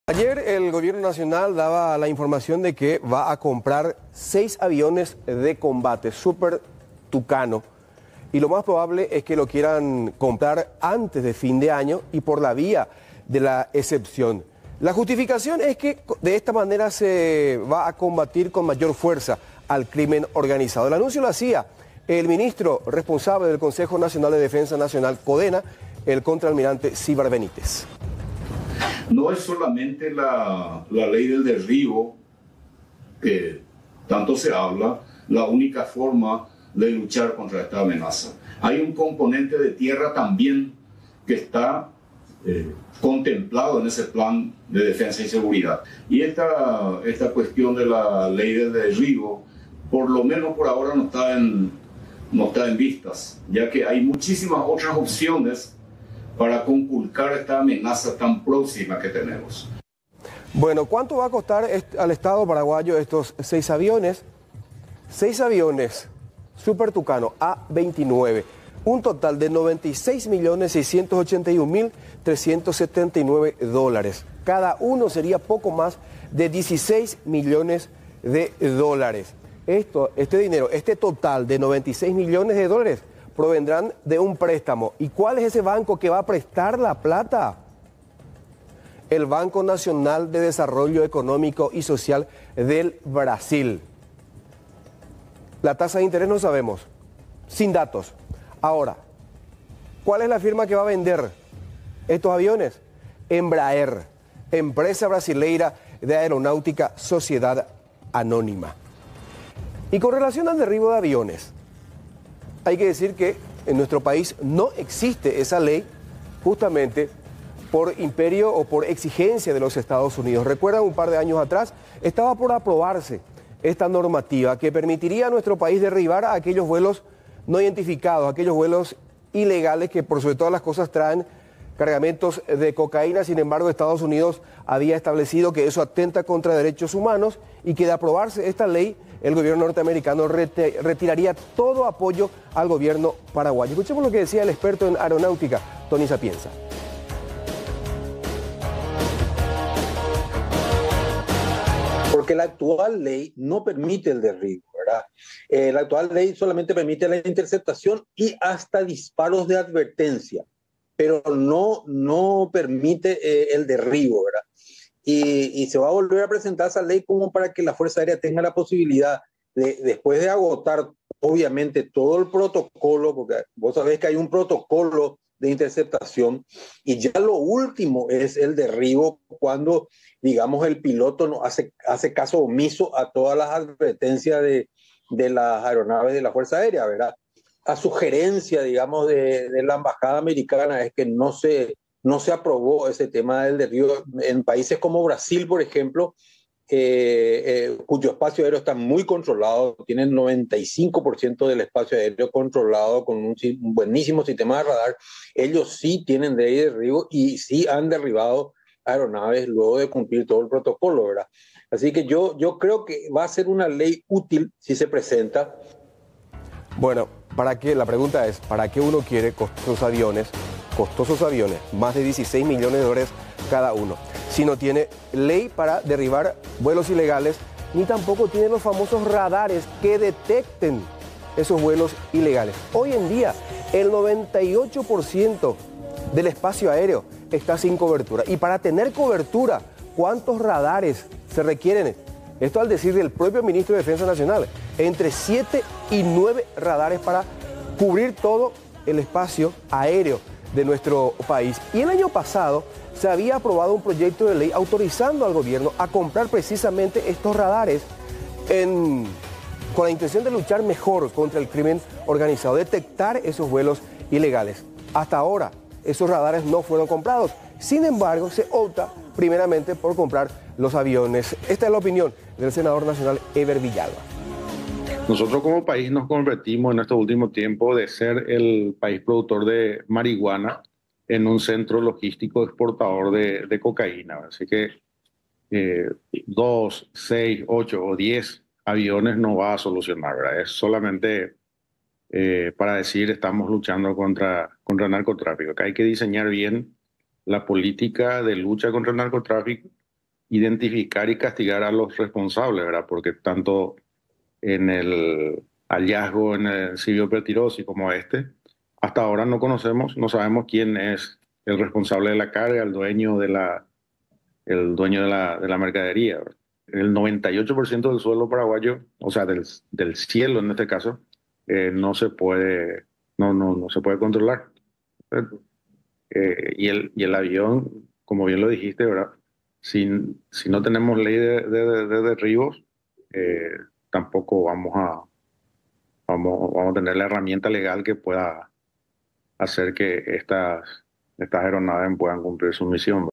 Estado comprará 6 aviones Súper Tucanos
El Gobierno realizará la compra de seis aviones de combate, los Súper Tucanos, con el fin de mejorar la seguridad nacional en apoyo a la lucha contra el crimen organizado. La adquisición se realizará por la vía de la excepción.

Para realizar la compra se deberá pagar con millonario préstamo: 6 aviones Súper Tucano A29, costará un total de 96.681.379 USD.